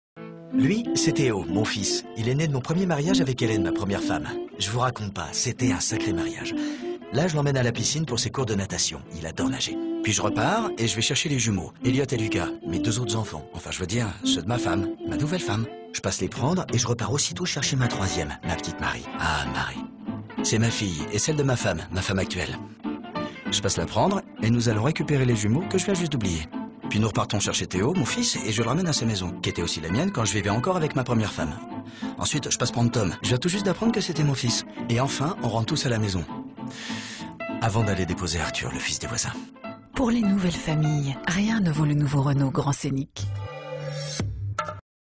Publicité Renault Grand Scénic - Famille recomposée.
La publicité raconte la famille.
Aujourd’hui, la notion de famille évolue. Il n’y a plus un seul modèle familial, mais plusieurs. Au-delà de la famille dite traditionnelle (papa, maman et leurs deux enfants) existent, en outre, la famille monoparentale, la famille recomposée, la famille homosexuelle…, autant de nouvelles familles qui marquent l’évolution de la société.

Conseil : Pensez à ralentir la vitesse de la vidéo sur Youtube (vitesse: 0.75) pour les plus bas niveaux.